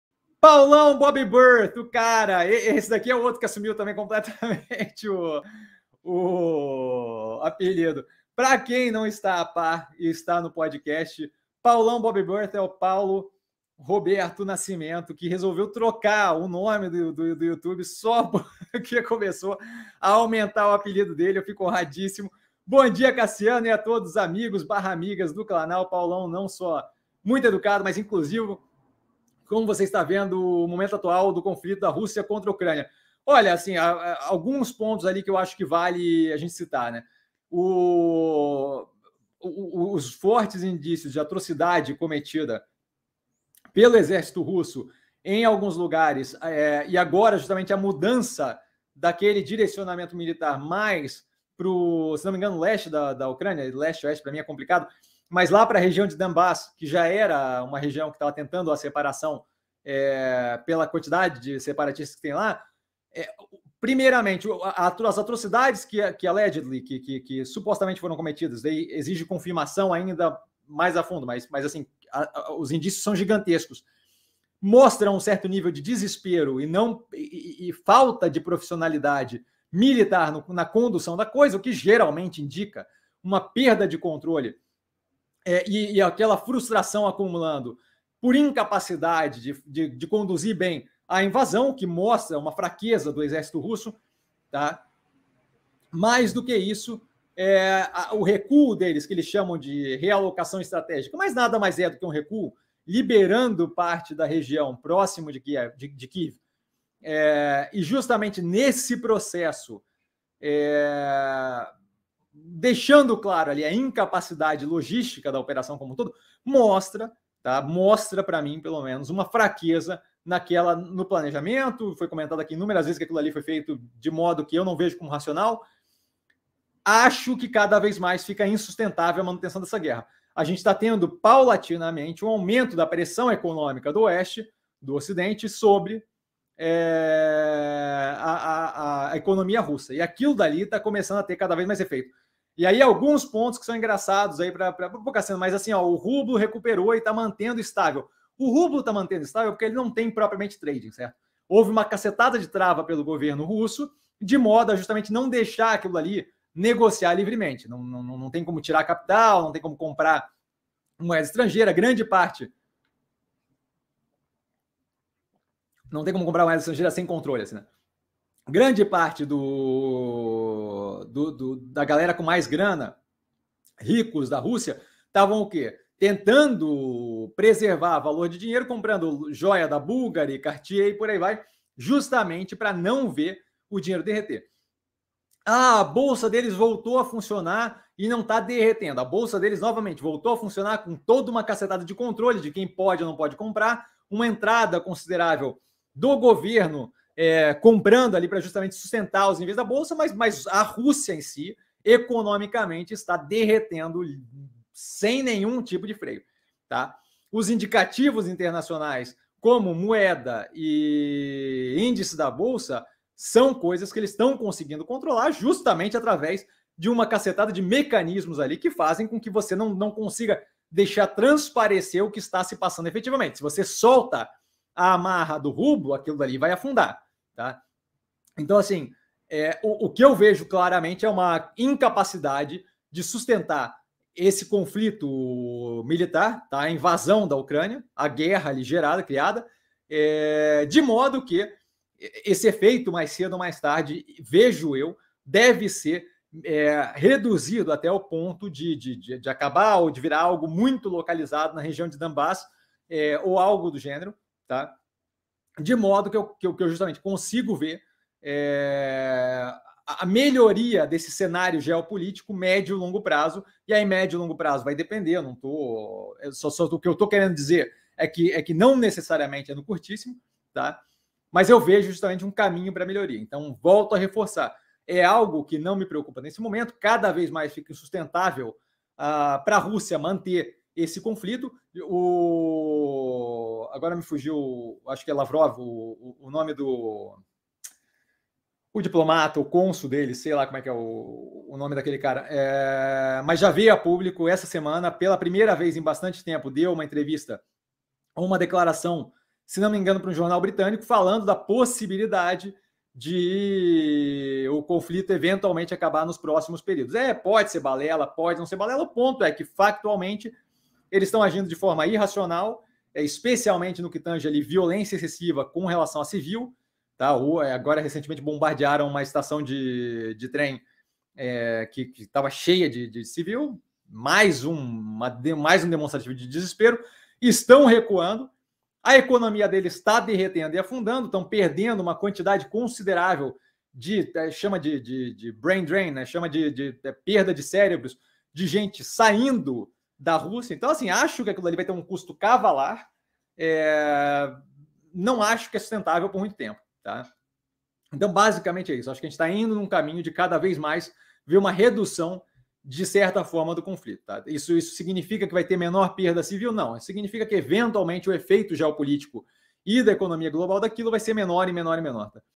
Como você está vendo o momento atual do conflito Rússia x Ucrânia? - Live by Warren 028 (09/04/22) -
Seleção das partes mais interessantes das Lives by Warren de sábado.